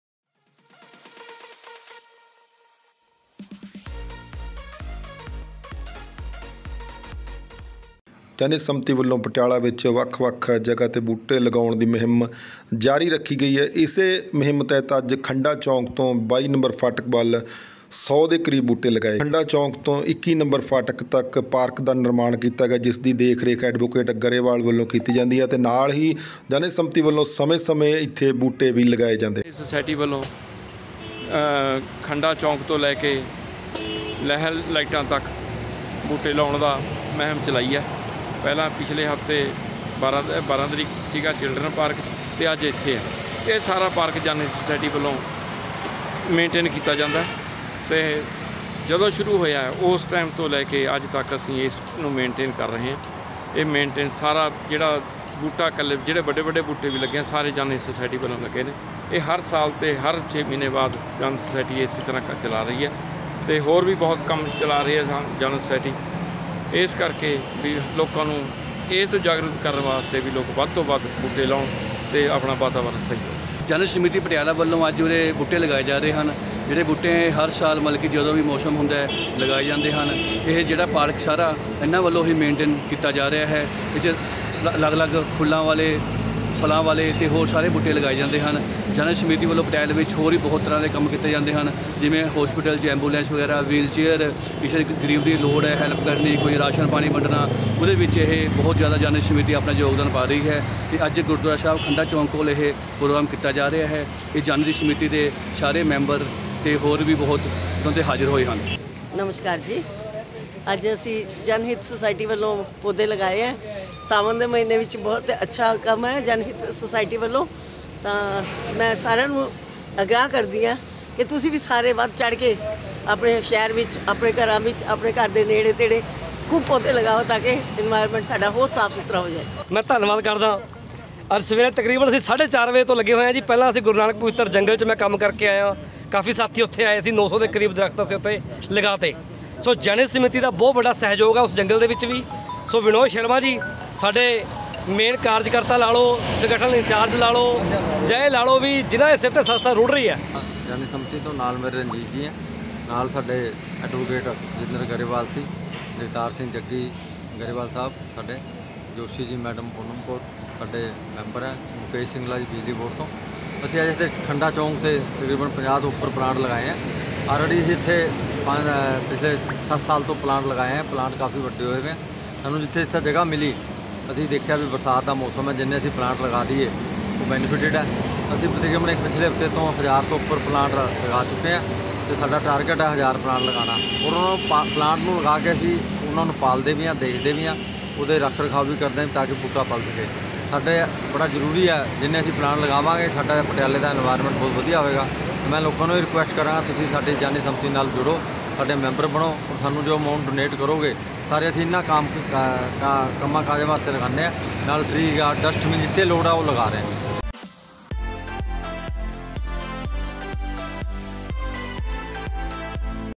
ਐਡਵੋਕੇਟ ਗਰੇਵਾਲ ਅਤੇ ਗ੍ਰੀਨਮੈਨ ਵਿਨੋਦ ਸ਼ਰਮਾ ਨੇ ਖੰਡਾ ਚੌਂਕ ਤੋਂ ਇੱਕੀ ਨੰ ਫਾਟਕ ਤਕ ਸੜਕ ਦੀ ਬਦਲੀ ਨੁਹਾਰ
Contact JatwinderGrewal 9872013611 ,RupinderKaur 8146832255
Please Like_Share_Subscribe
Youtube--- StringsNews & FaceBook--@Strings News